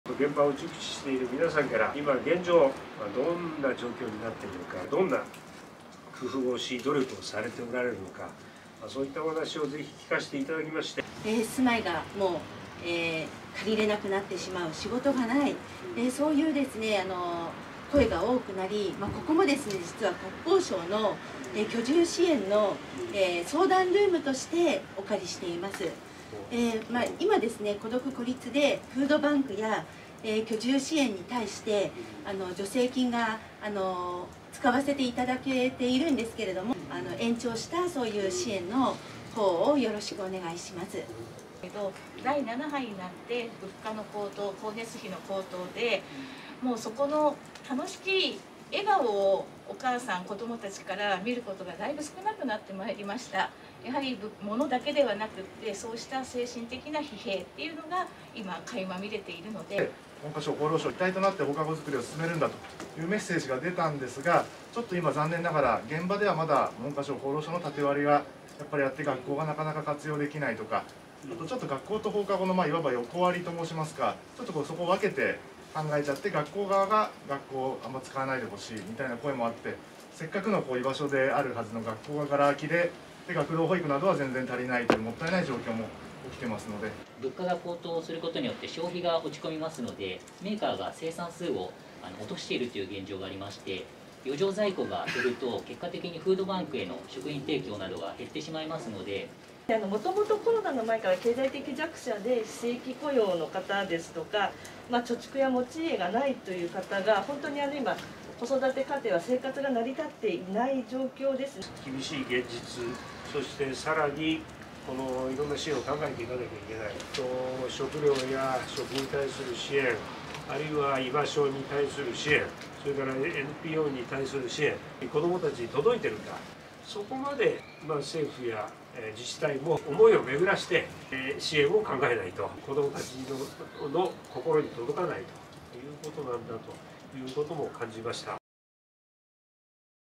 経済的困難を抱える家庭の子どもたちの支援者との車座対話―令和4年10月11日
経済的困難を抱える家庭の子供達の支援に最前線で奮闘されている皆様と意見交換しました。切実な現状や更なる取組の必要性をお伺いしました。本日の御意見も踏まえながら、今月中に取りまとめる総合経済対策において、支援に万全を期してまいります。